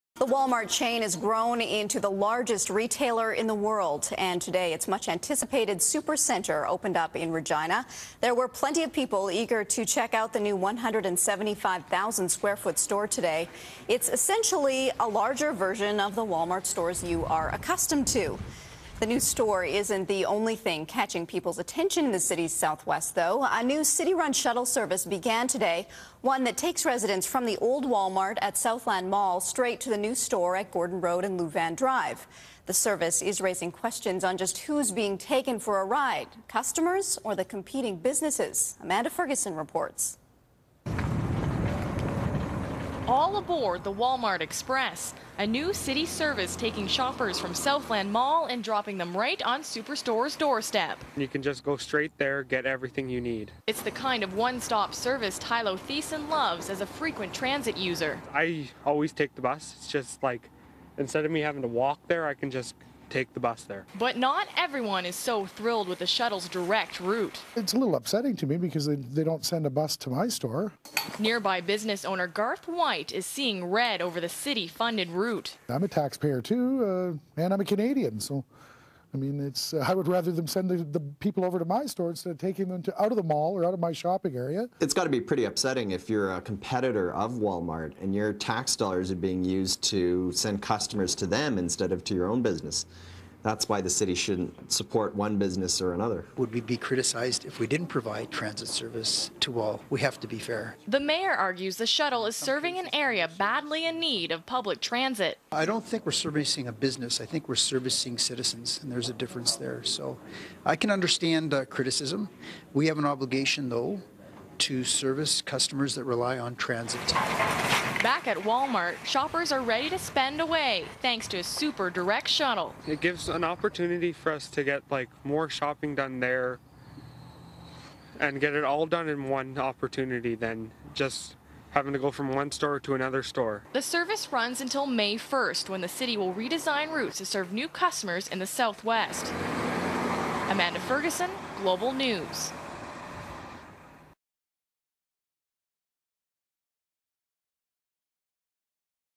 Taxpayers Fund Walmart Express
The City of Regina is funding a new shuttle that runs every half hour from Walmart's former location in the Southland Mall to its new location further west. Other businesses and Colin Craig of the Canadian Taxpayers Federation say this practice shows favoritism. Mayor Pat Fiacco says the goal is service to citizens, not businesses. Roughly two-thirds of Regina's transit budget comes from taxpayers.